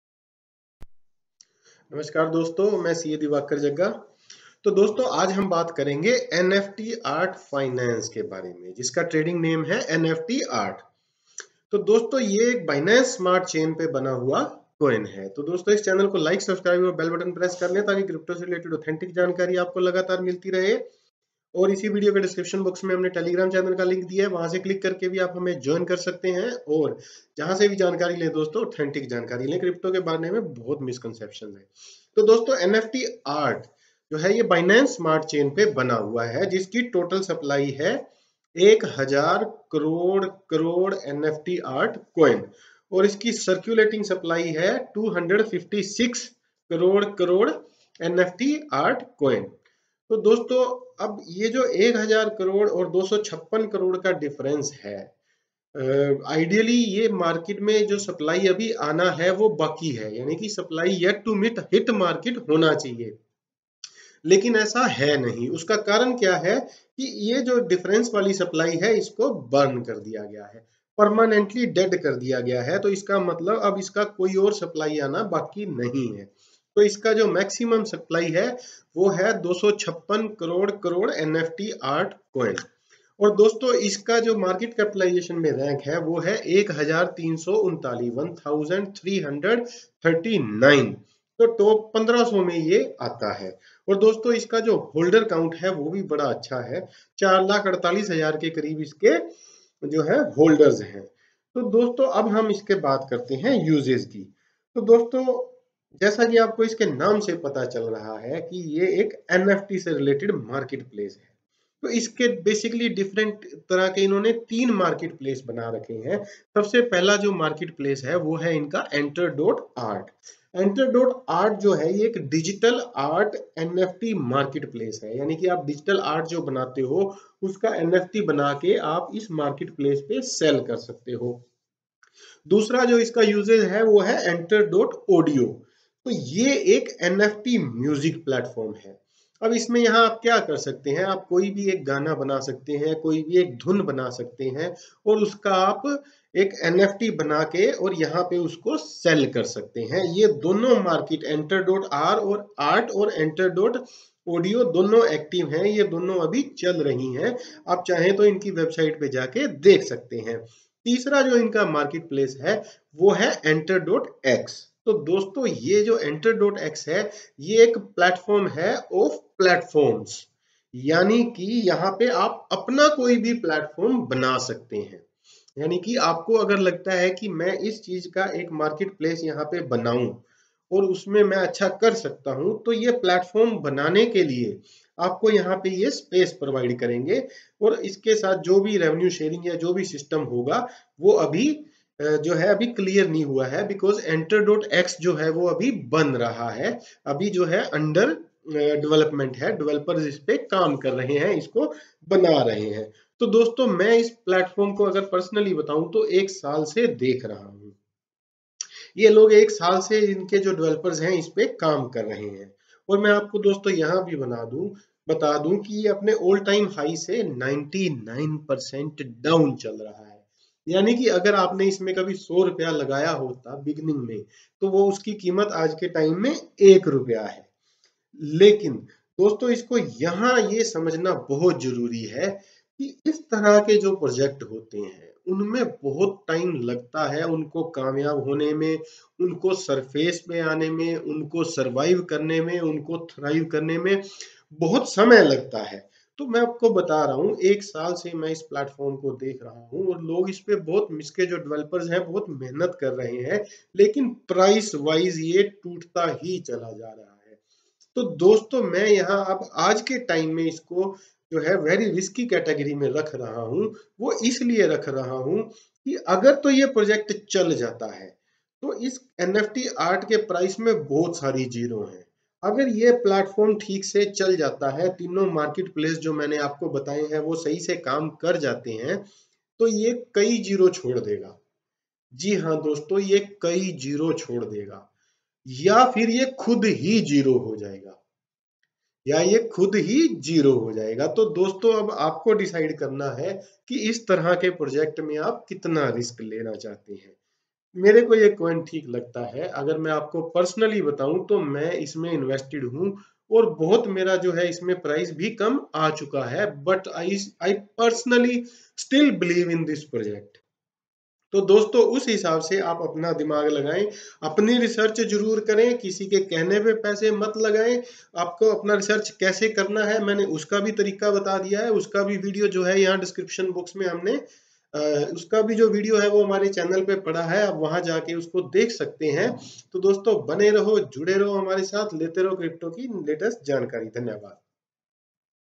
🎤Price Prediction 💸
nftart finance has three different nft marketplace , two are operational and one is under development, check all bout this here.

CA Divaker Jagga is having 18 years of experience in the finance and 10 year of experience in the financial markets.

We did an in depth study of the cryptocurrencies, how they evolve, how they work, what is the logic behind their emergence and  what will be the future of these financial instrument.